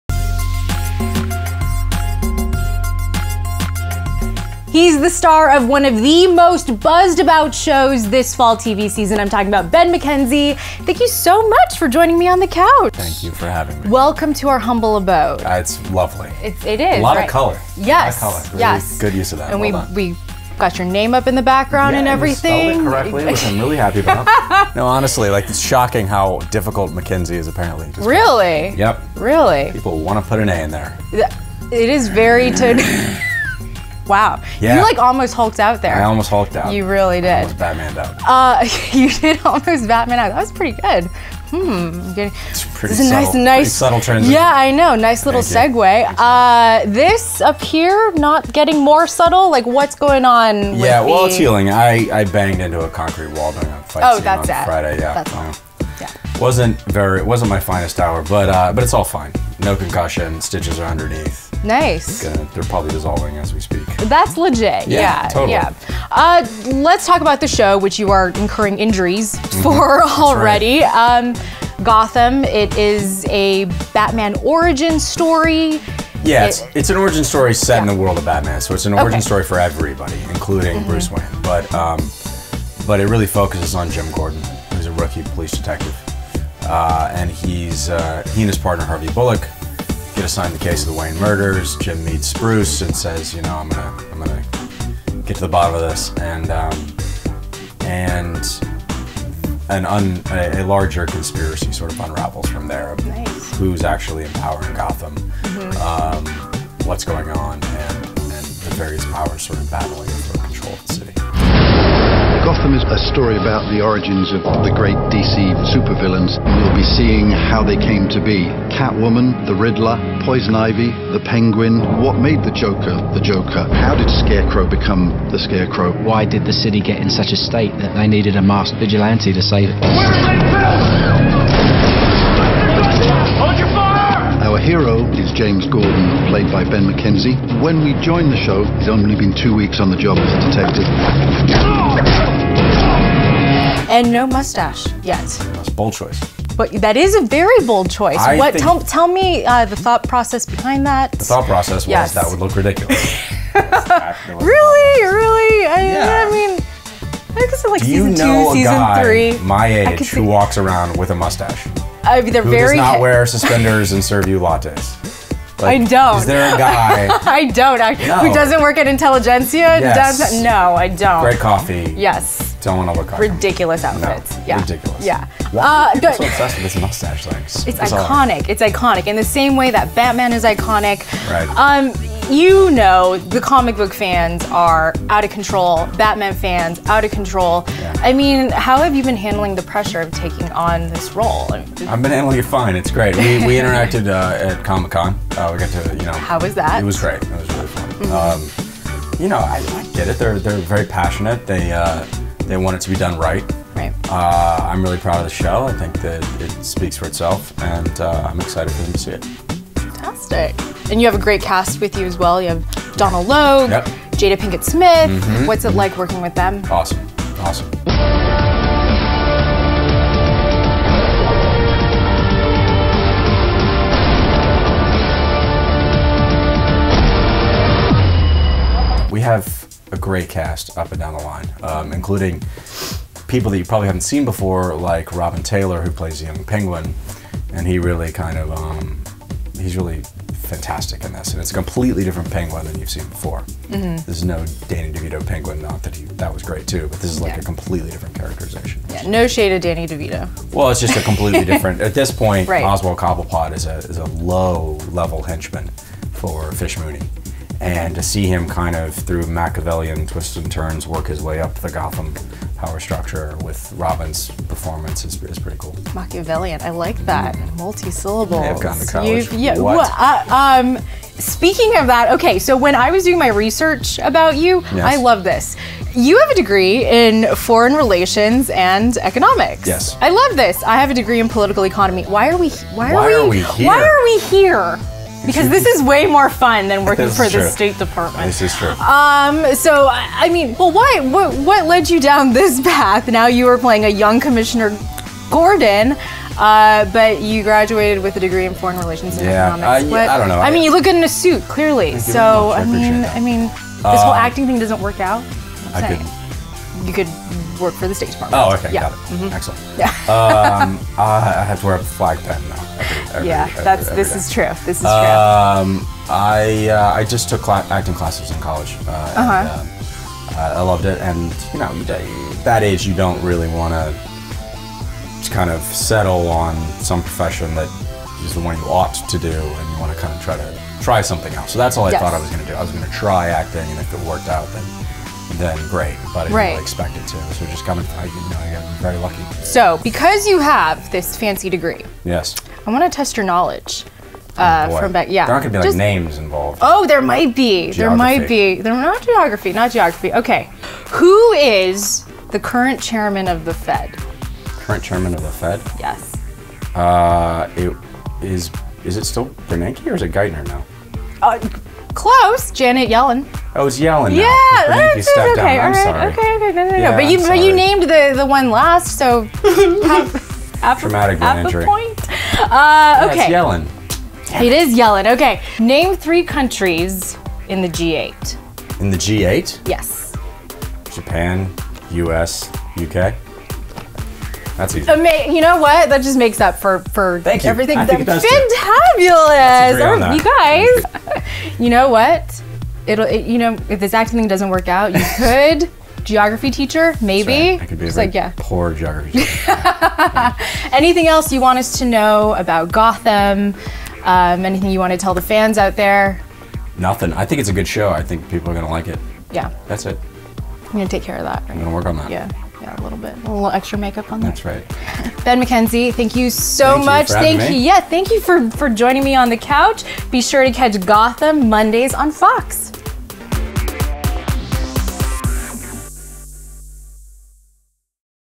Ben McKenzie on GOTHAM's Batman Universe Origin Story!
"Gotham" star Ben McKenzie pays a visit to the YH Studio to chat with us about his role as the iconic Gotham City commissioner-to-be, James Gordon, in Fox's Batman universe origin story series! He even shows off a battle wound from the show, plus he explains why he decided to ditch Gordon's equally-iconic mustache! We also quiz him to see how much he remembers from his foreign relations and economics studies in college!